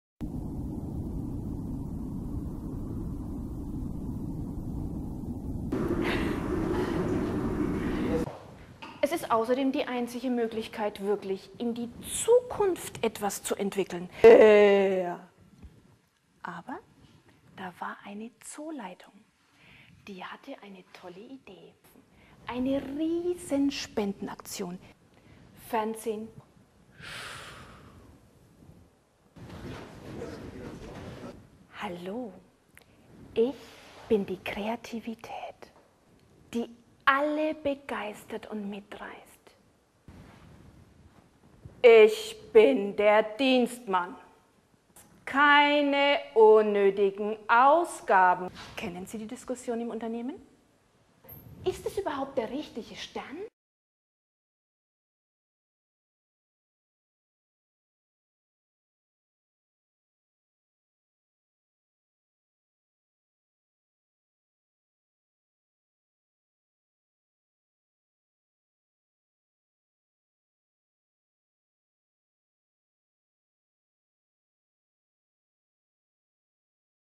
Kreativität - denken, spinnen, Kunden gewinnen
Live-Mitschnitte aus dem Vortrag "Kreativität - denken, spinnen, Kunden gewinnen" von Alexandra Herzog-Windeck